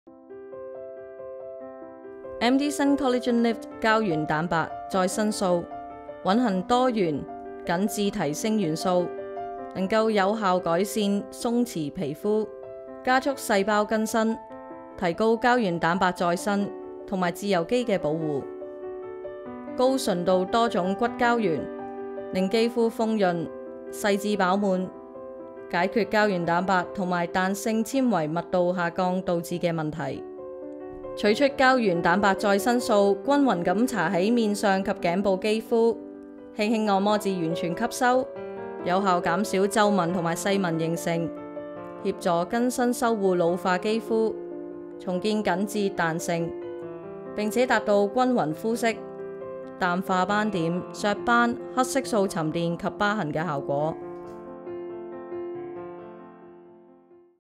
MDSUN 妍心賞 Collagen Lift 膠原蛋白再生素 (廣東話)
MDSUN 妍心賞 - 膠原蛋白再生素 (又稱 超級黃金再生膠鈦)

蘊含多元胜肽再生因子及豐富 DNA 幹細胞，以尖端科技結合 24K 奈米黃金的「突破性超級奢華抗老配方」！

瞬效拉提，深層除皺！媲美 「電波拉皮」改善鬆垮肌膚，重建立體緊實線條效果顯著，同時豐富的 Oligo 寡肽微量元素、維他命及氨基酸，能幫助促進細胞更新，防止真皮層纖維細胞鬆垮，刺激膠­­原蛋白增生和自由基保護。

高純度多重膠原蛋白，讓肌膚豐潤，細緻飽滿，解決膠原蛋白及彈力纖維密度下降導致的問­­題，有效減少皺紋及細紋形成，協助更新修護老化肌膚，重建明顯緊緻與彈力度，並且達­到­均衡膚色，淡化斑點﹑雀斑﹑ 色素沉澱及疤痕的效果。

無與倫比的全方位護理卓越功效，是一款令肌膚找回“童顏肌膚” 的高效濃縮凝膠再生精華。

-- 減少及預防眼周和額頭部位的表情紋及靜態紋
-- 迅速激活肌膚，促進細胞新陳代謝
-- 刺激彈性纖維增生，膠原蛋白再生
-- 撫平細紋、增強肌膚柔嫩平滑度
-- 改善鬆垮，恢復肌膚緊緻彈性
-- 改善粗糙及皺紋，延緩肌膚老化
-- 改善毛孔粗大及皮膚的發炎狀況